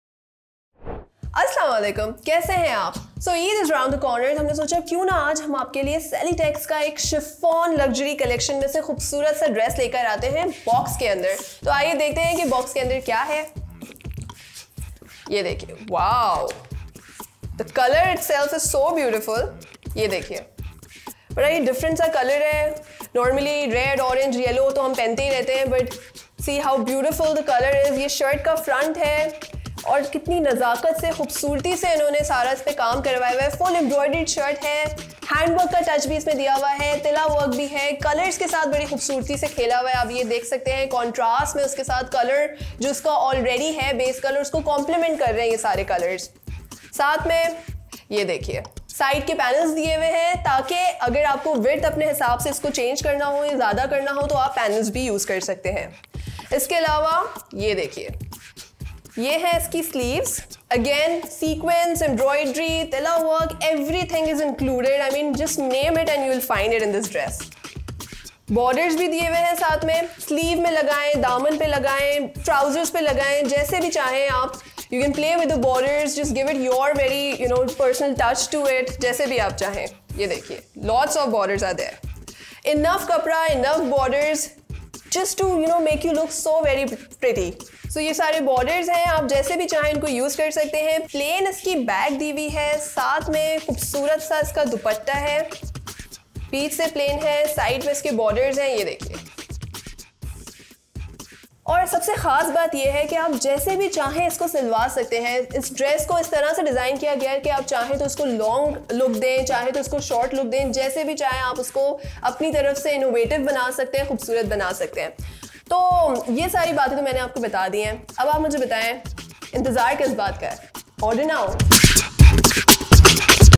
HC 00007-Salitex Festive Luxury Chiffon
Look glamorizing & fabulous with Salitex Festive luxury Chiffon
Price Starting from 6450/-
Product Description:
Embroidered chiffon front with hand work
Embroidered chiffon side panels
Plain chiffon back
Embroidered chiffon sleeves
Embroidered chiffon attachment laces
Embroidered chiffon dupatta
Raw silk trouser